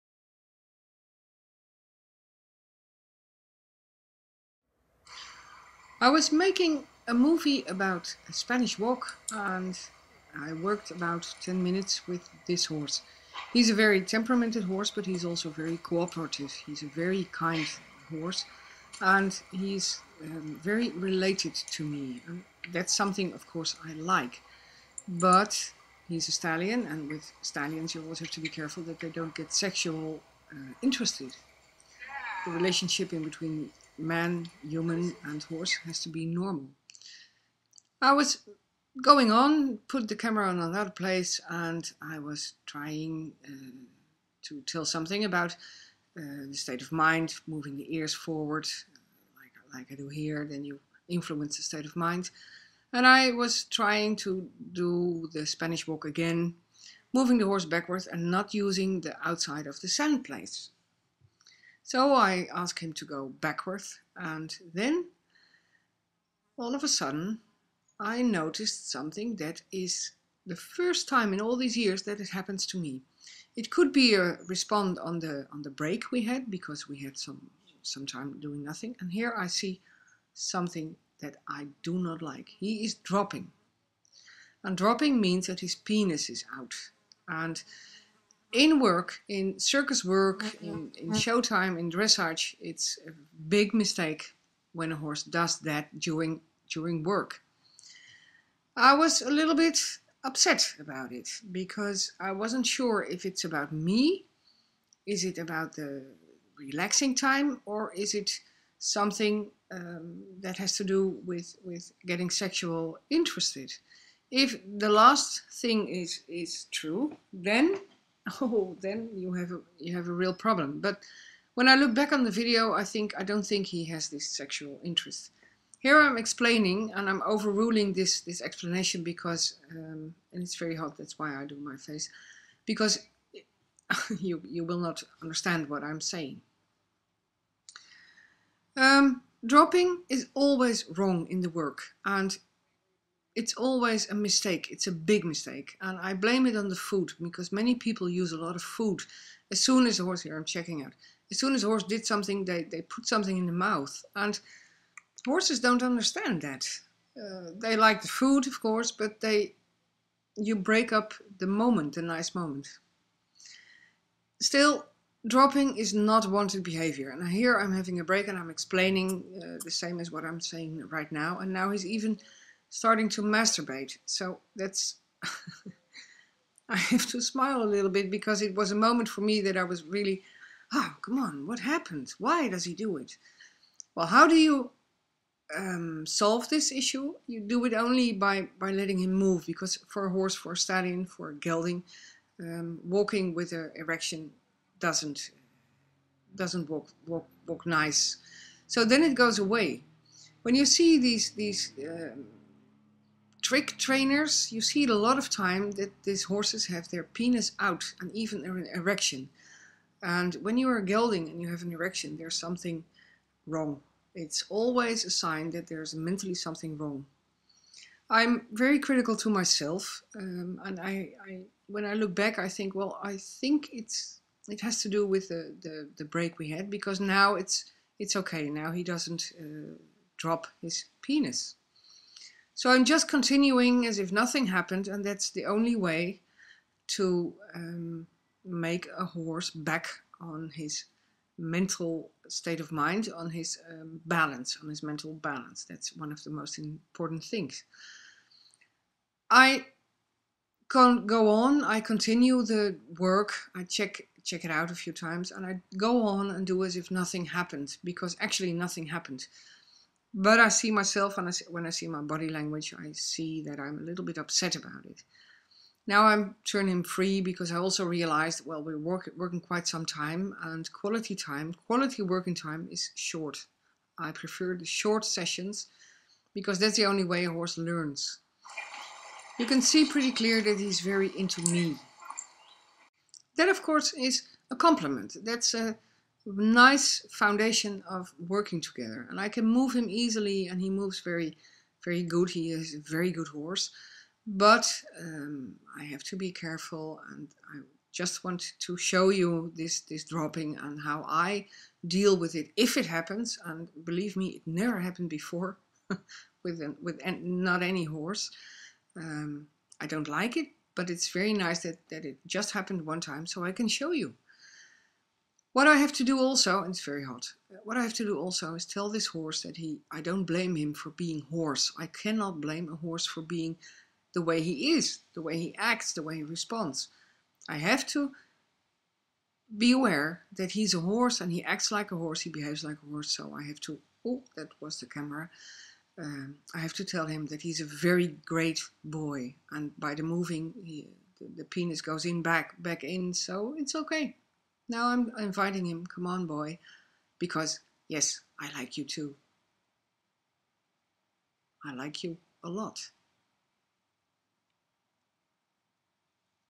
Dropping during work-time - Horse letting his penis hang out
Horses drop when they have to pee or when they are relaxed. Normally no Horse will walk with his penis down and flapping around, and when it happens.

Dropping means that a horse drops his penis while he is at work.
Dropping during the work should be a clear sign for every trainer and rider that something is wrong.
It is always a bad sign and tells us that he is mentally confused and that is caused by humans. No male horse will drop or have an erection during the work.

Its a small mistake in training, you see it many times in clicker training or food focussed training. The Horse gets fed after he shows the wanted behavior, but he can not figure out what the human trained is really doing. That is what causes the dropping during work.

For stallions its is more normal to drop, they use the penis to masturbate but normally not in the neighborhood op people. If it is because you are a woman and activated the sexual need then there is something to work on! Its very dangerous, and a stallion who wants to mate a human can cause bad damage.

As soon as the stallion is getting sexual interested, which is not normal and very unhealthy you should correct that.Snap the Horse out of the situation and walk him, that will help in most of the cases. Imagine what happens when a horse jumps at you, that is very dangerous. Because if he jumps at you and gives his 'love bite' in the neck the chance of being badly injured is big.

For geldings it always means a mental unbalance, be aware of that and watch careful what your horse does. It is sad for a gelding to walk with an erection, its not good. They only drop when they are standing still and are relaxing or when the have to pee. Sometimes you see a gelding half way dropped and even then it is a sign of confusion.

In all other circumstances there is something wrong.

Remember when it happens in your performance, or during dressage or jumping people will laugh because its a trainers mistake. When it happens at dressage competition with a stallion, you will be eliminated because the horse is not concentrated and not in balance.